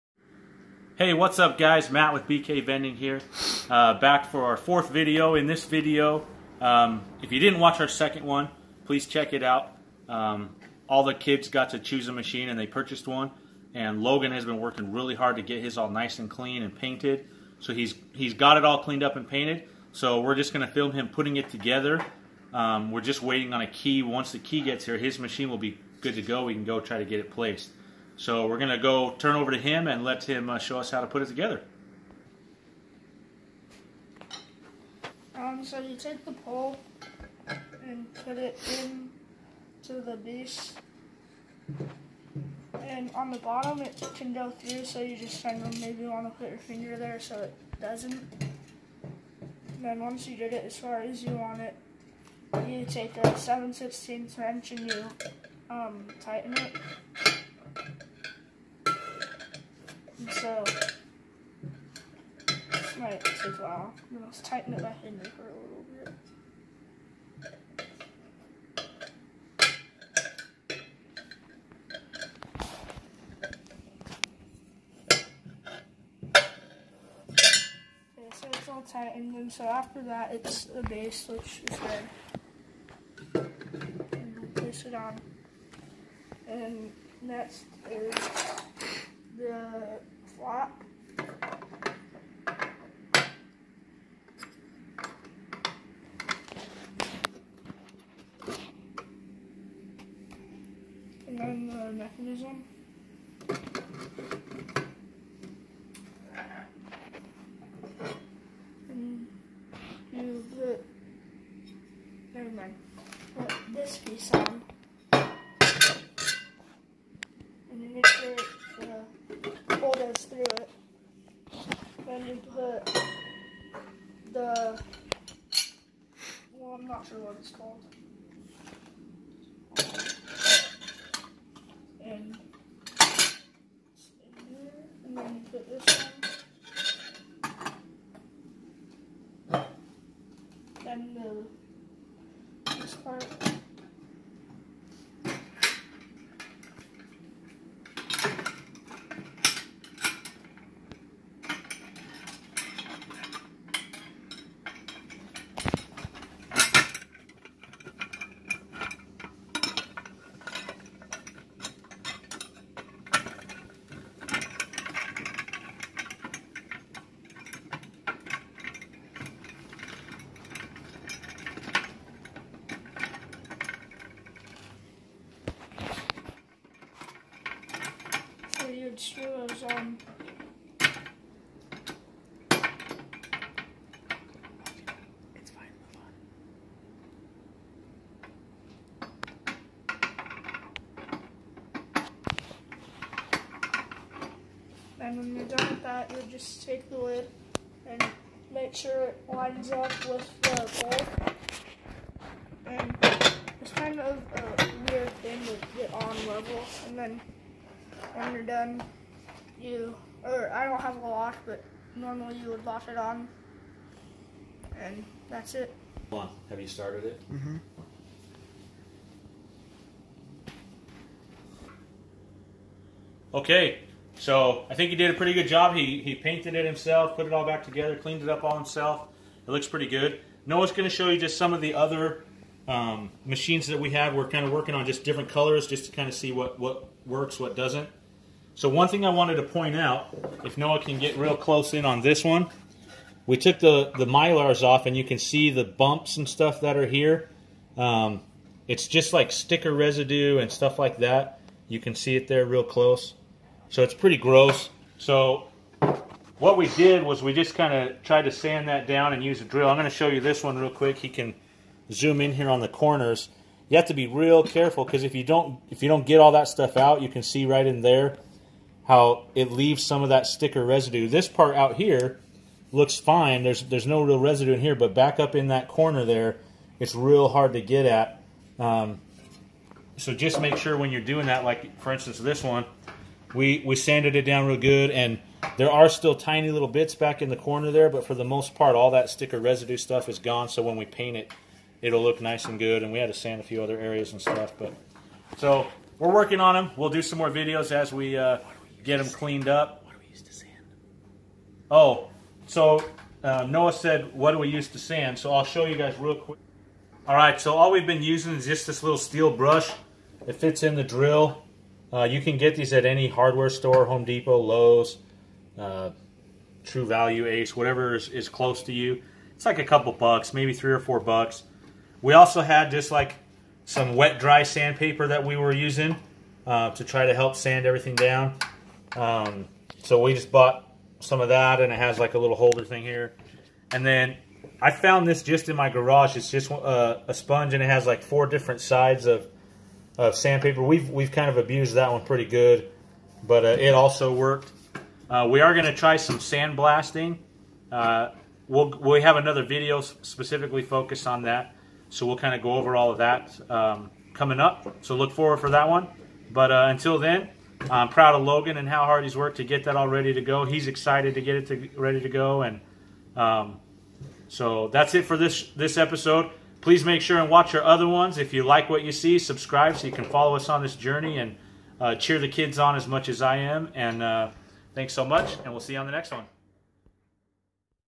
How to Put a Beaver Gumball/Candy Machine Together
Logans machine is ready and he puts it together.  10 years old and we're ready to place it.

Want to learn more about vending, check out my vending Family and Friends below